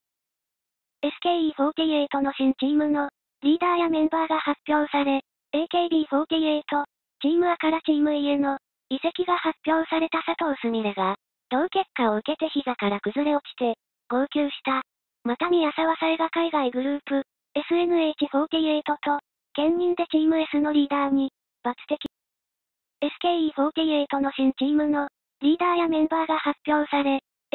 佐藤すみれ【号泣】AKB→SKE移籍で泣き崩れる～AKB組閣 FULL HD
AKB48 team846人のメンバーを一挙紹介の動画 『AKB48グループ大組閣祭り』（24日、東京・Zepp DiverCity） SKE48の新チームのリーダーやメンバーが発表され、AKB48・チームAからチームEへの移籍が発表され